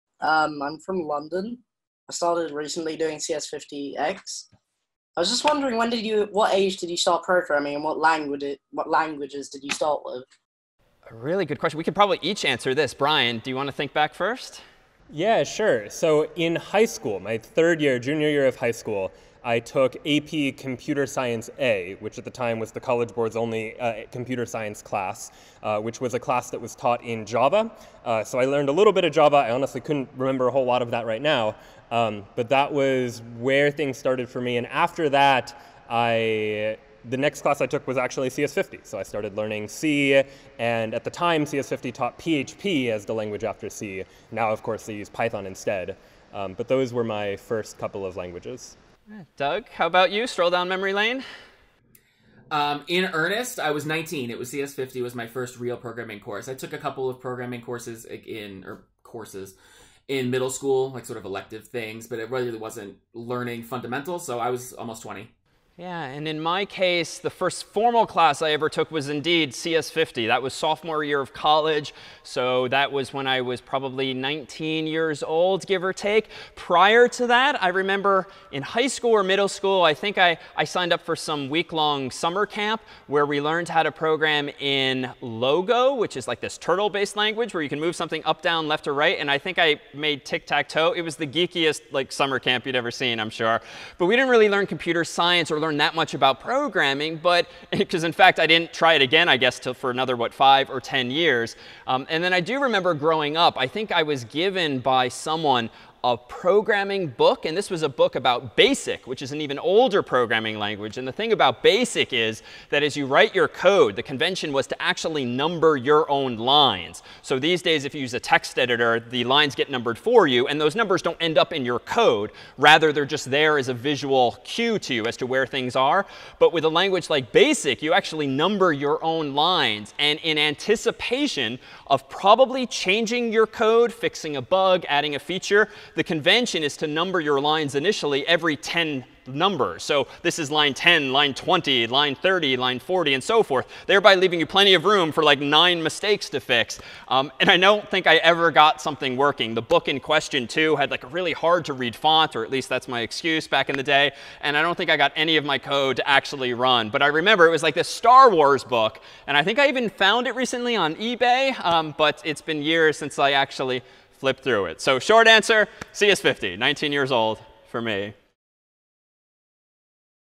At what age did you start programming? - CS50 Office Hours 2020
Office hours are an opportunity for students online to ask questions about any of CS50's courses, questions about computer science, questions about programming, and the like.

In this clip: At what age did you start programming and what languages did you start with?

CS50 SHOP

LICENSE